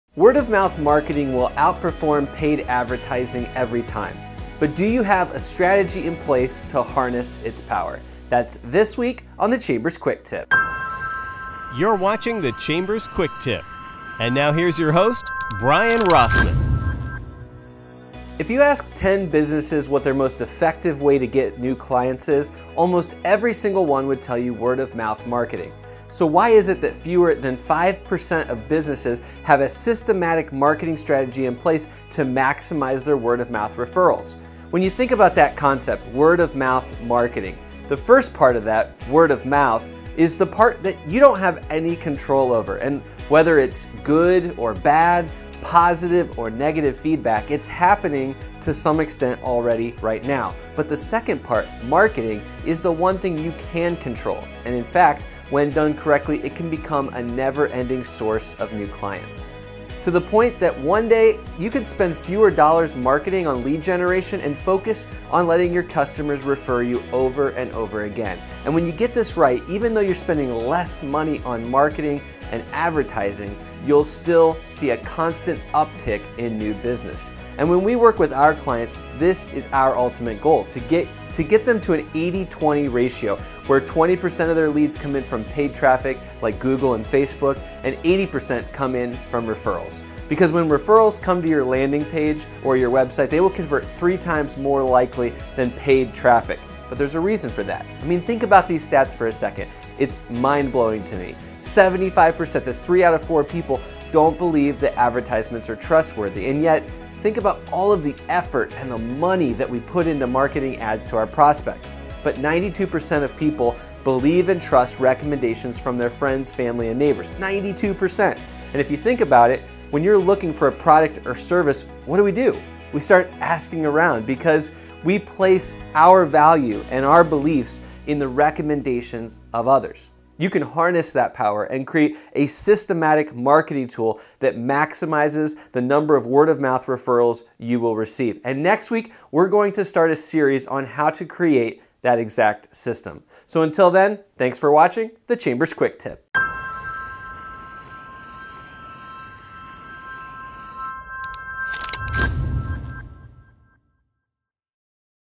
Word-of-mouth Marketing
This week on The Chamber’s Quick Tip, we start a series of episodes talking about word-of-mouth marketing.  Learn why word-of-mouth marketing beats paid advertising every single time.  What have you found to be the best way to encourage word-of-mouth referrals?